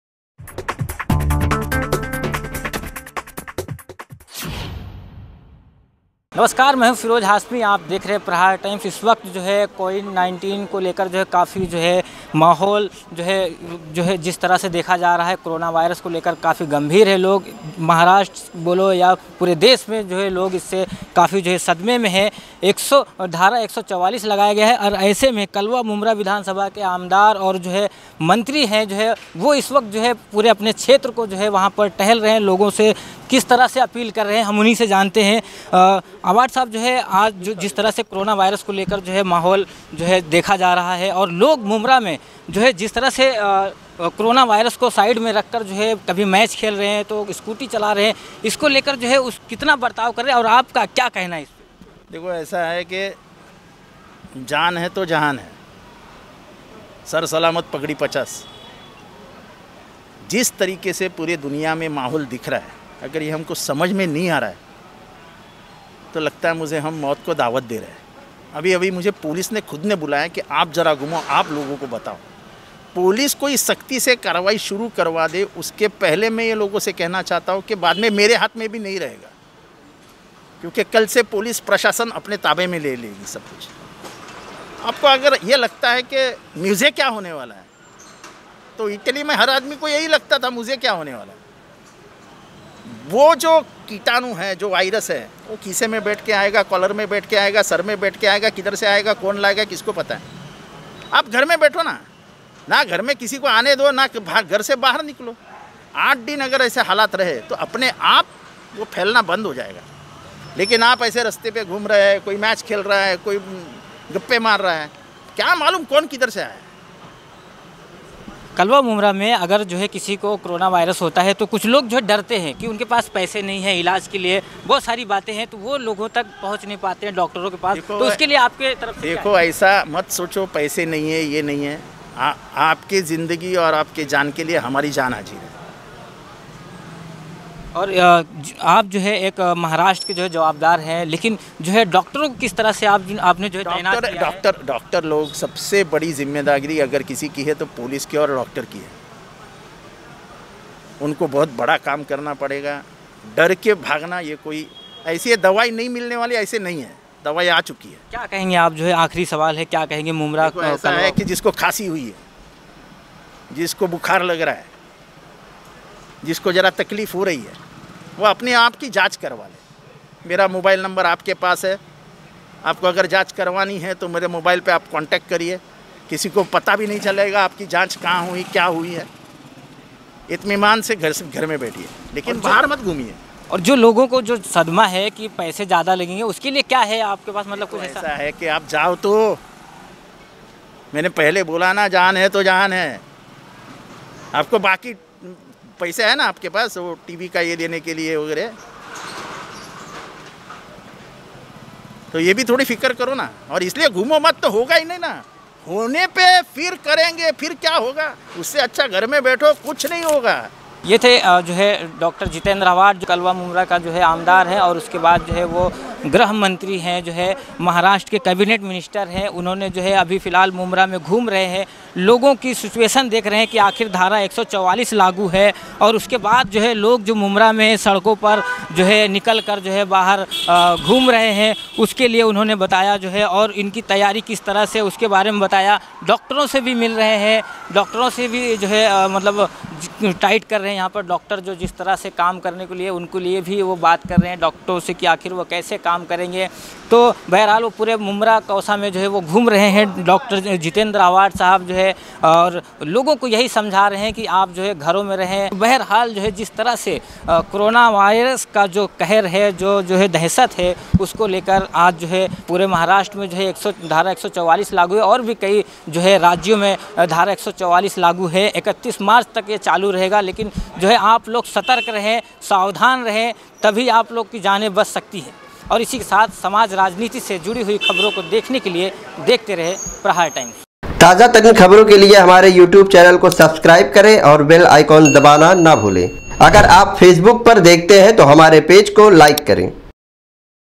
मंत्री जीतेन्द्र अव्हाड ने लोगो से किया अपील कोरोना से बचना हैं तो घर में रहो | firoj hashmi Times
मंत्री जीतेन्द्र आह्वाड ने लोगो से किया अपील
कोरोना वायरस से बचना हैं तो घर में रहो